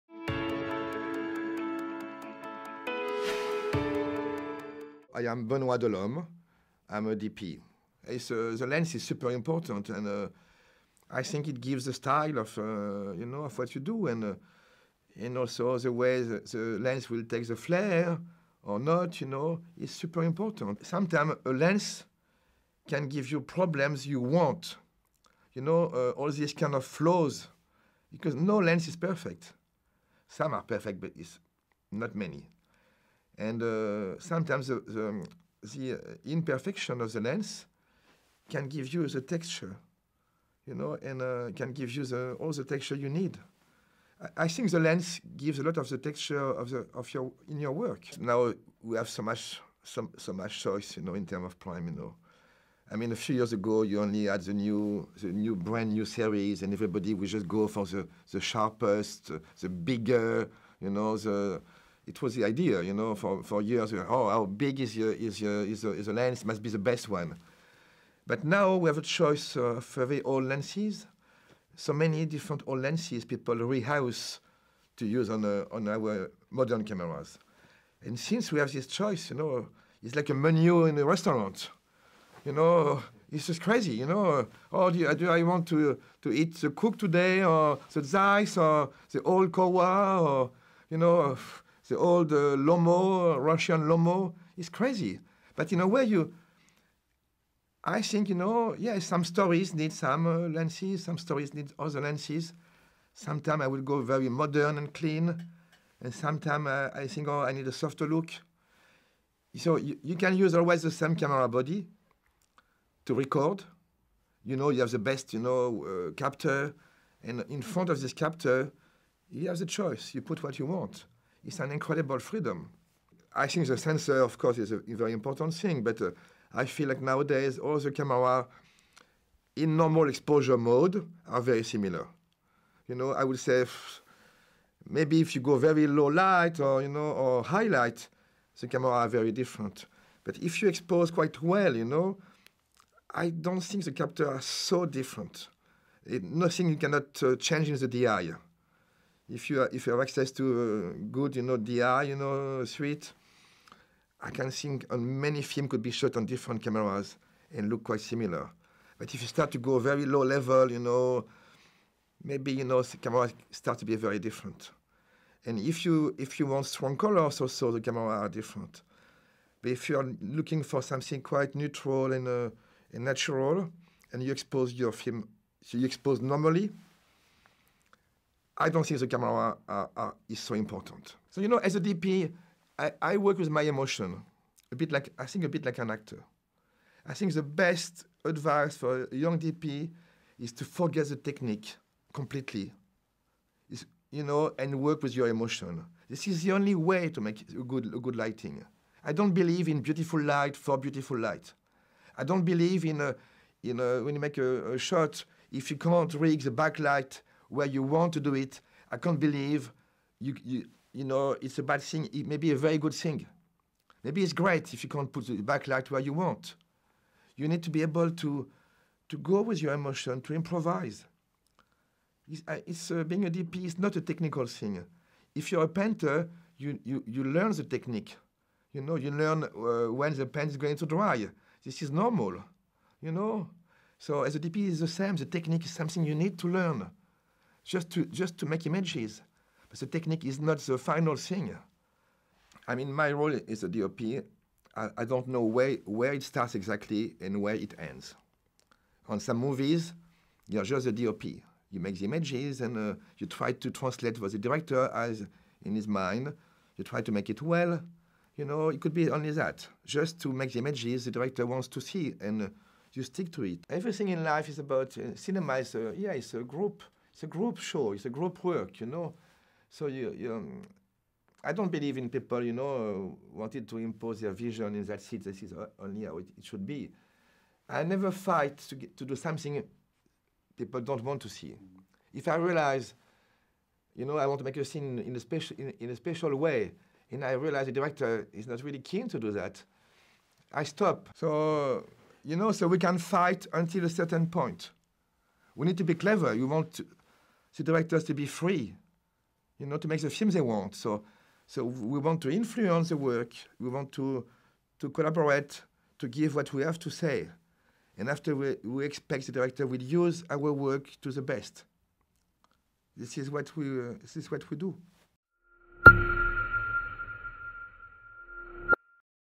Talking Cinematography: Benoît Delhomme AFC
In this episode of "ZEISS presents: Talking Cinematography" cinematographer Benoît Delhomme AFC talks about the importance of a lens, lens choices in modern cinematography and about working with the director.